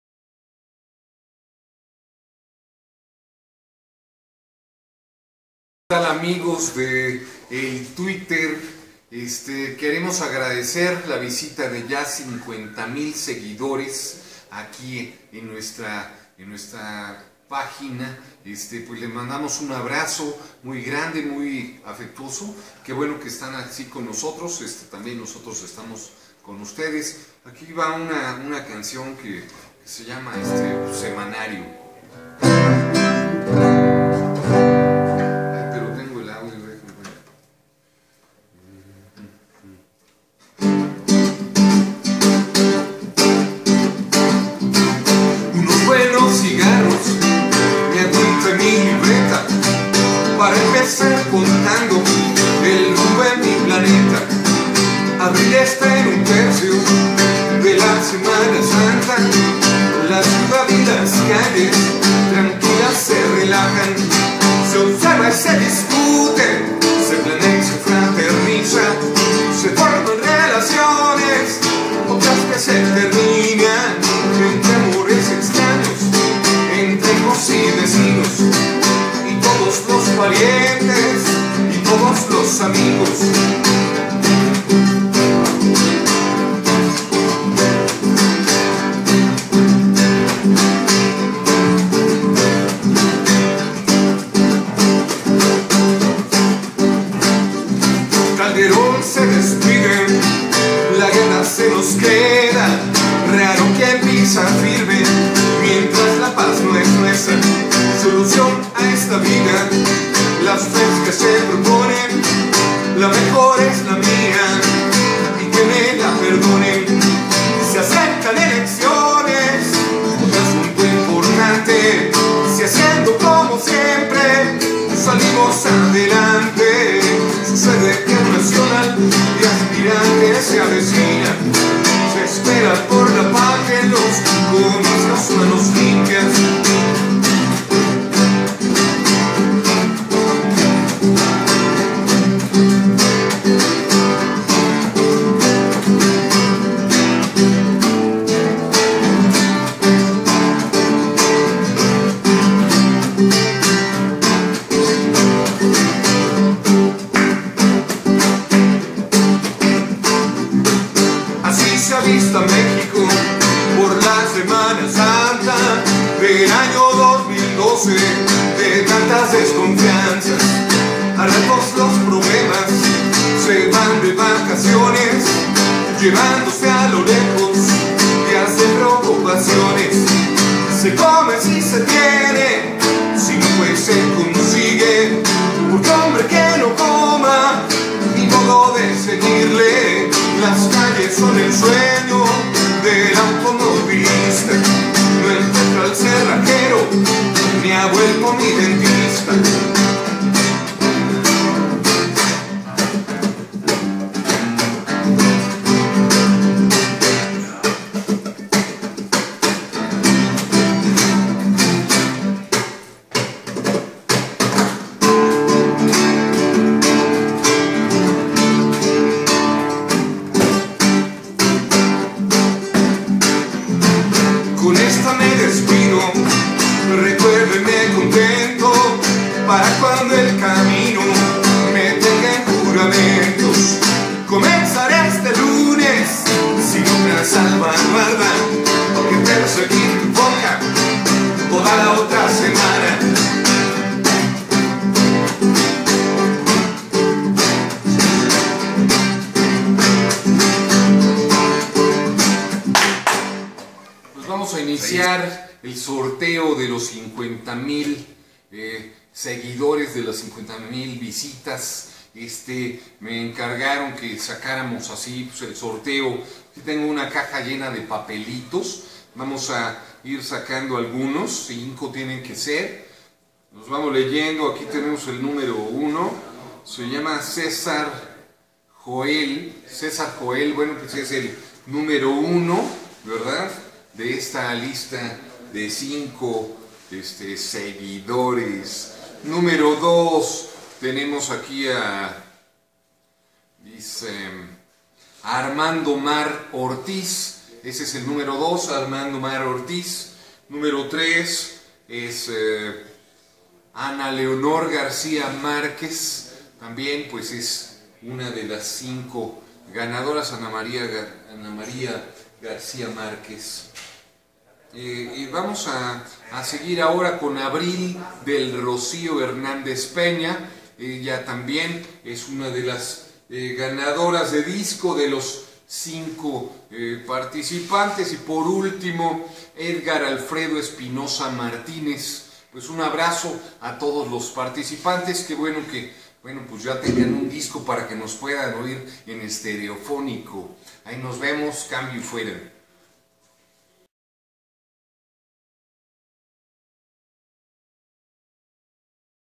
Para celebrar 50,000 seguidores en Twitter
Hace unos días alcanzamos los 50,000 seguidores en Twitter y aprovechamos para regalar unos discos y cantar una canción.
El número es sólo un pretexto, pero agradecemos muchísimo que siempre anden compartiendo con nosotros.
Abrazo grande,
FD